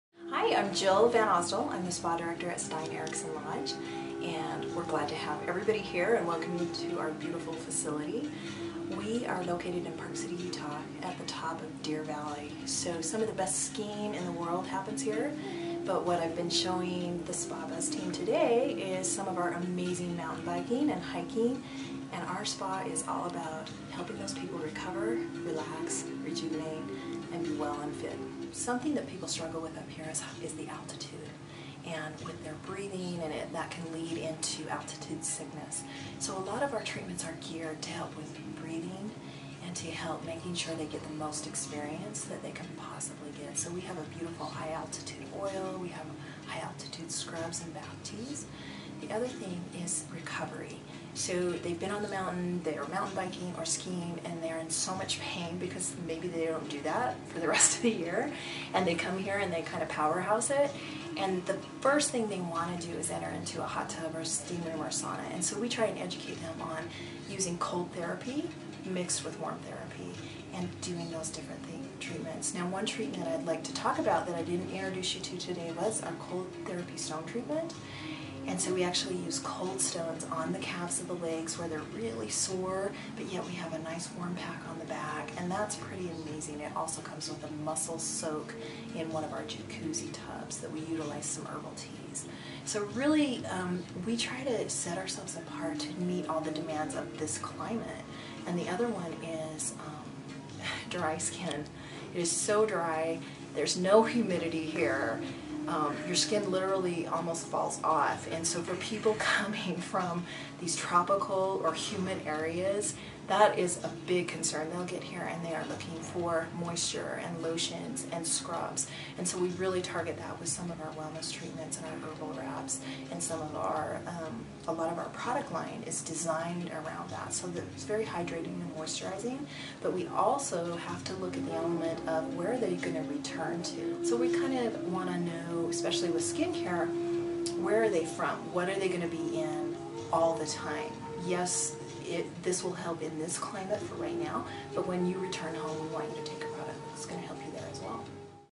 Stein Eriksen Lodge
Jill VanAusdal, Spa Director for Stein Eriksen Lodge explains the treatments that The Buzz Bus roadies received and the focus of her team on educating guests about the high altitude Park City environment.